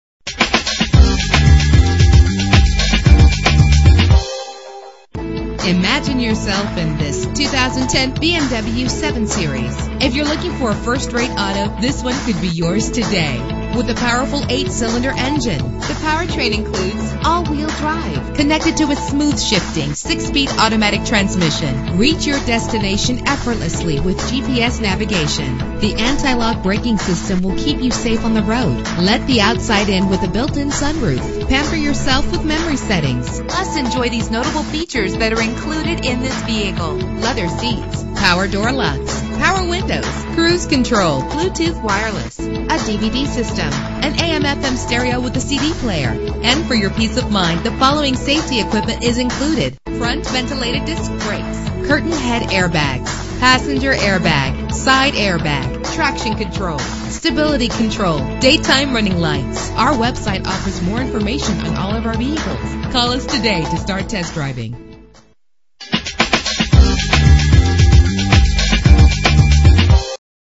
2010 BMW 7 Series - Bedford OH
Year: 2010
Make: BMW
Model: 7 Series
Trim: 750Li xDrive
Engine: 4.4 liter 8 cylinder 32 valve
Transmission: 6-Speed Automatic
Color: Mineral White Metallic
Mileage: 32948
Address:
333 Broadway Ave